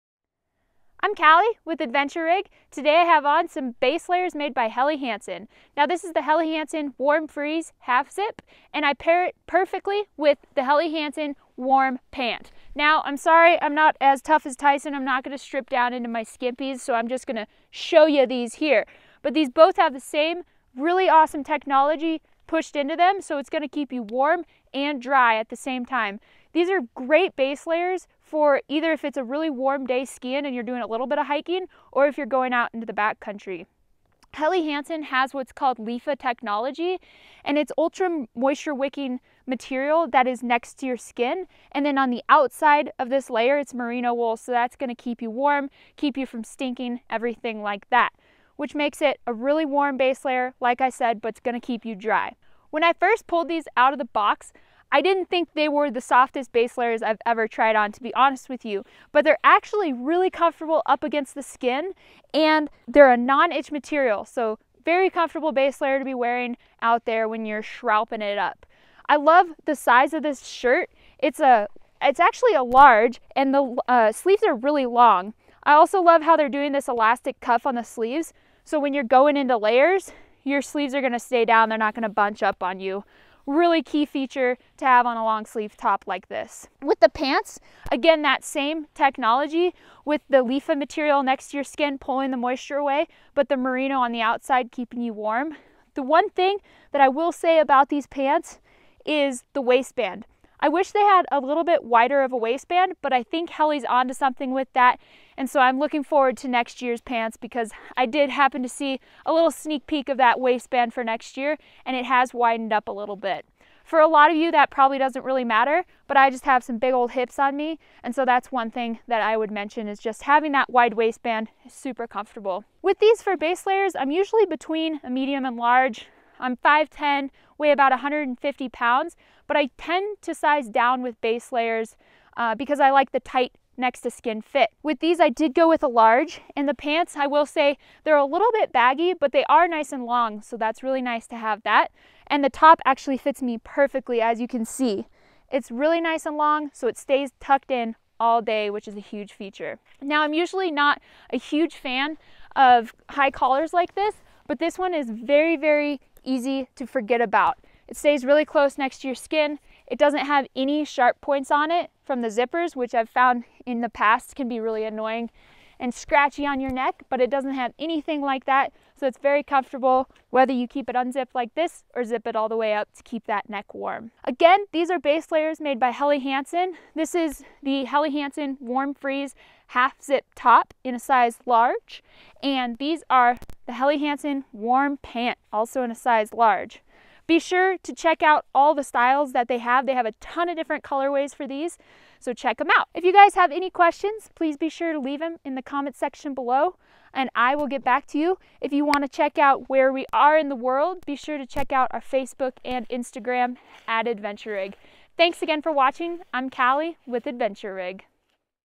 Women's Helly Hansen HH Base Layers [Review]
Women’s HH Warm Freeze ½ Zip

Helly Hansen combines all of the best technologies in this ½ zip base layer that is ideal for cold snow sports and out performs when you’re working at high aerobic activities. The HH Warm Freeze ½ Zip will keep you warm and dry while being a comfortable, well fitting base layer.

The Warm Freeze is very soft and keeps you warm thanks to the use of pure merino wool. With that, you get merino properties such as anti-microbial features, extreme softness to the touch, and a very non-itch wool. Personally, merino wool is the number one fabric that I can have in or as a base layer when I’m doing anything—whether in cold temperatures or not.

Combined with the merino, Helly adds in their Lifa Stay Dry Technology, which has been around for a long time and has proven to keep you dry time and time again.   With the Lifa fiber next to skin, you are guaranteed to stay dry especially if you are working up a sweat in the backcountry.

Aside from the technology going into the Warm Freeze, overall it fits really well and won’t rub your skin raw thanks to flat lock stitching. The sleeves are plenty long, which I sometimes have an issue with, and I love the elastic-ish cuffs. Not only do they stay where they’re supposed to, they don’t ride up when you go to layer over them. It’s not something that you’d really think about with a base layer but once you realize it, it’s amazing!

I’m not normally a fan of ½ zip base layers (I generally opt for crew) but with this layer, I hardly notice the zipper. It’s almost like a hidden zip and very small and compact so it won’t get in your way when you layer up over it.

If you’re looking for a base layer that is going to keep you comfortable, warm, and dry, look no further than the HH Warm Freeze ½ Zip. It’s easy to tell and feel that this base layer was built for a women’s body and you’ll notice that the second you slip it on. This top pairs perfectly with the HH Warm Pant so check out those details below!

Women’s HH Warm Pant

Helly Hansen combines some amazing technologies and fibers into the HH Warm Pant making it the perfect base layer bottom for any of your cold weather endeavors. Whether you're lapping the steeps and deeps at the resort or skinning in the backcountry, the Warm pant will keep you warm and dry.

Merino wool is by far one of my favorite fibers to have in a base layer especially when I’m skiing. It’s extremely warm, super soft, and has great features like being anti-microbial so you won’t start to stink after your 3rd or 4th wear. Helly Hansen went ahead and improved a simple merino base layer by using merino as an insulating layer but putting their Lifa Technology next to your skin to keep you dry.

Together, the merino wool and Lifa Technology work to keep you warm and dry no matter what you are doing. Flatlock stitching ensures comfort no matter what you are layering up over your Warm Pant.

One thing that I don’t like about the Warm Pant is the waistband (which I believe Helly is changing in 17/18). It is just about an inch thick and pretty noticeable during wear. I am a huge fan of a wide waistband as I feel as though it makes base layers nearly unnoticeable (as they should be). With the small waistband, I feel as though these just aren’t nearly as comfy as they could be.

As far as sizing goes, I would definitely recommend sizing down if you are kind of in between sizes. I’m normally a large in outerwear (esp. for the length) but always hesitate when I go to order base layers. Usually I stick with medium but in some cases, I feel as though they may be too tight and too short. Also, if base layers aren’t truly next to your skin, they can have a hard time doing their job. I went with the large in these and definitely should’ve gone with the medium! The larges are extremely long on me and just don’t fit me like a base layer should.

Whether you’re looking for a base layer or an outer layer, Helly Hansen has what you need. Paired perfectly with the HH Warm Freeze ½ Zip you’ll be warm and dry out there with the Lifa and Merino combo.

Links